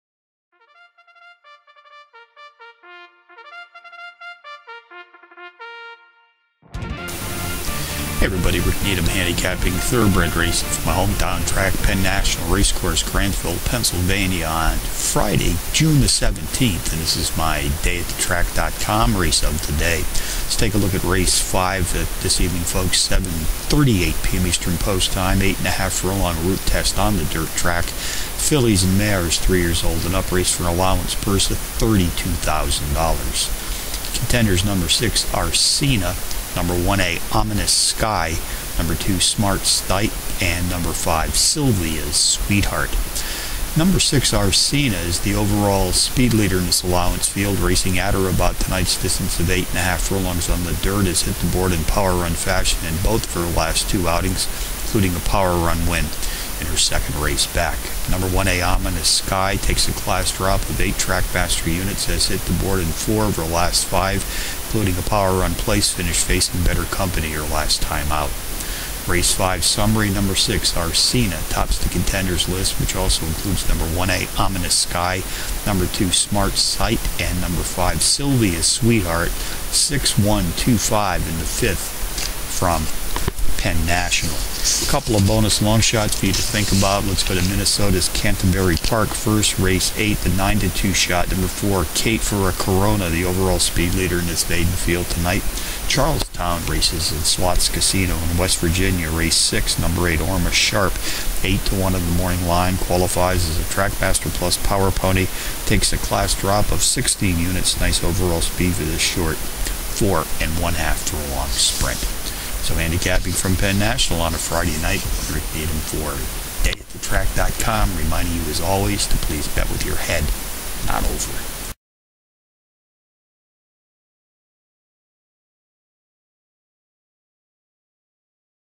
pennational0617.wmv
Race #5 - Penn National Race Course - Friday June 17, 2011 + bonus longshots from both Canterbury Park & Charles Town